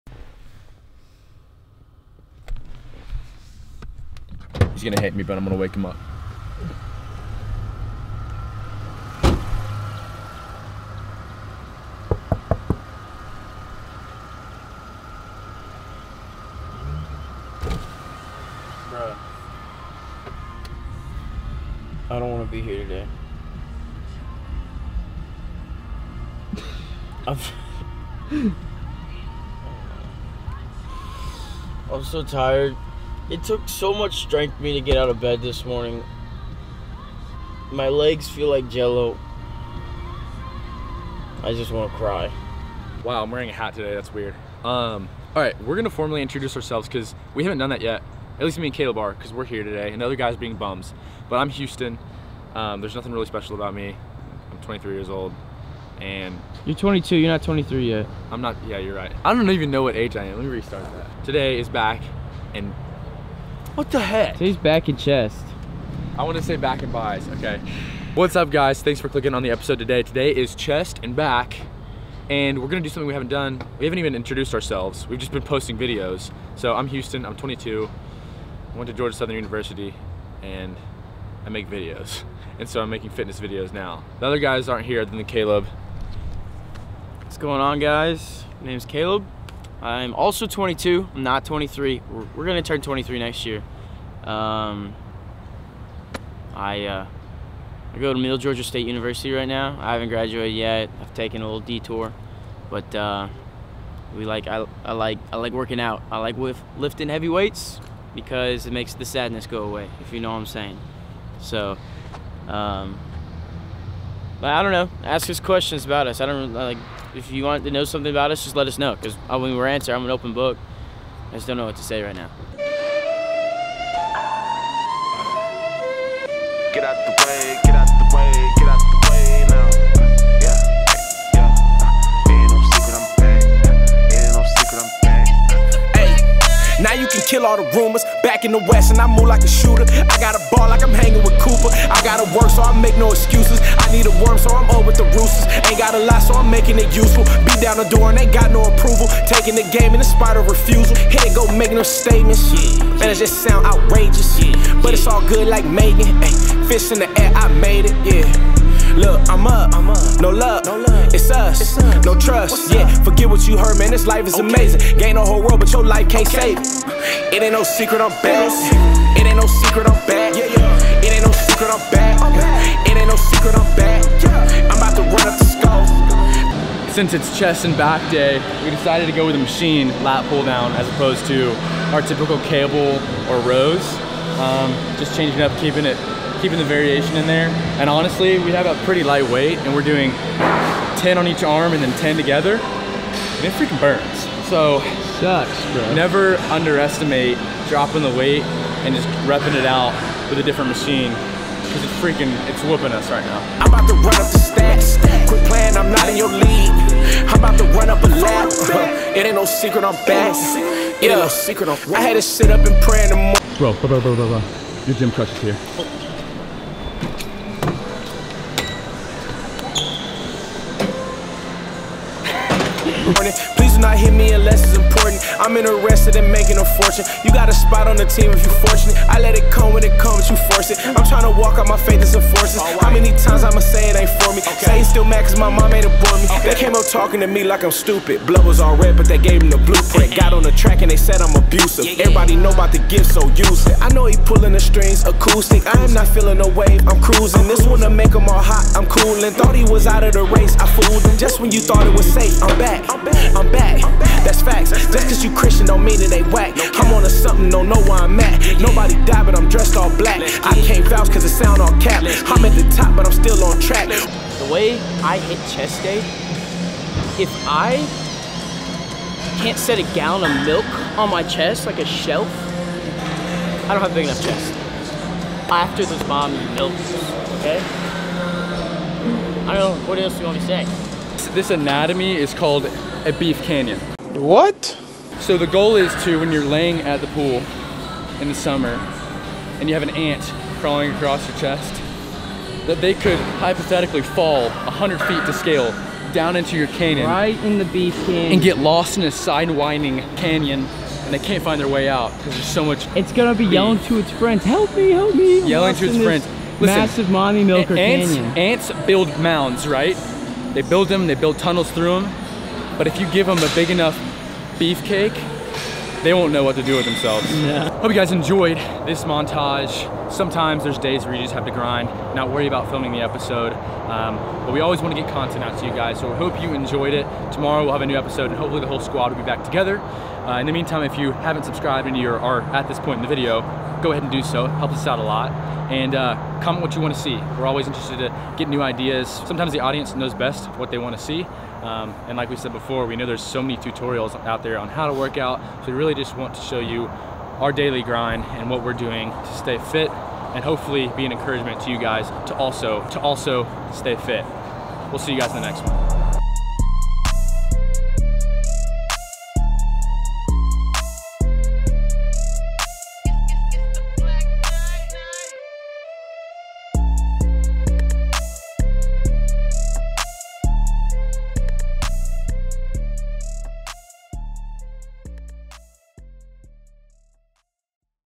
How to Build a Sturdy Shelf | CHEST DAY
Caleb and I hit a chest and back workout together today. It was a head down and focus kind of day. We also explain the importance of building a shelf-like chest. Enjoy!

MB01SAHR2DTNHUU